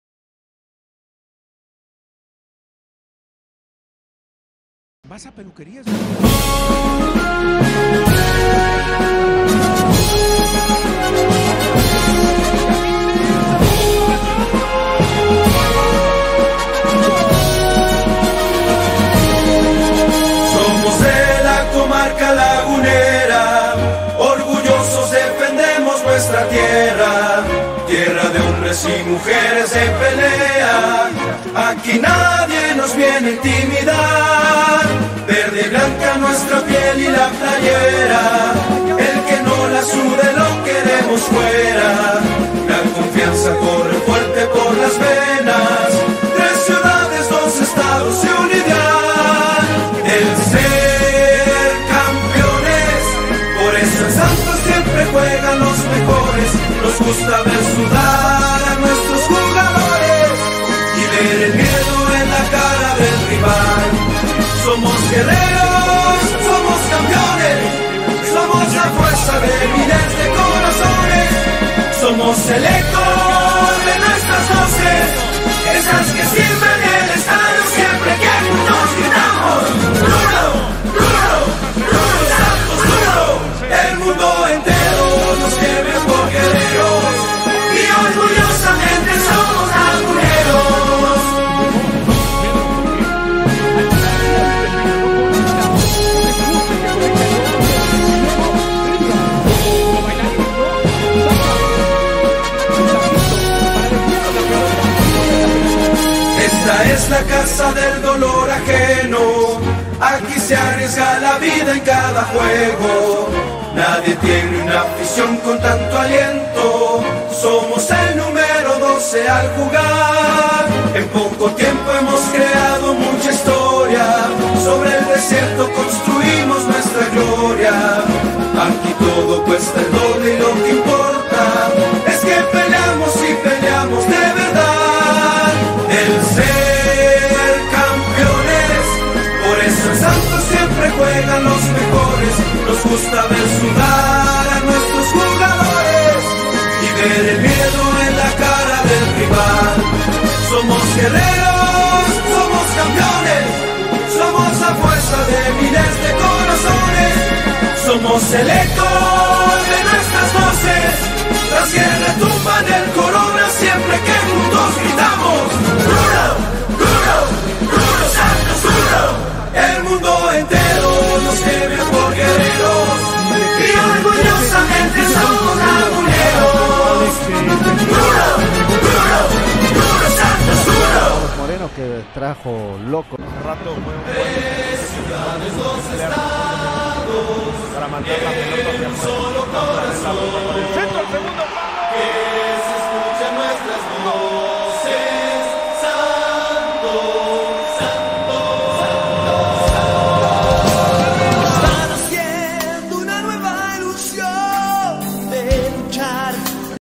GRACIAS ULISES | TODOS LOS GOLES DE ULISES RIVAS EN SANTOS LAGUNA
Revive los 5 goles de Ulises Rivas con la playera del Santos Laguna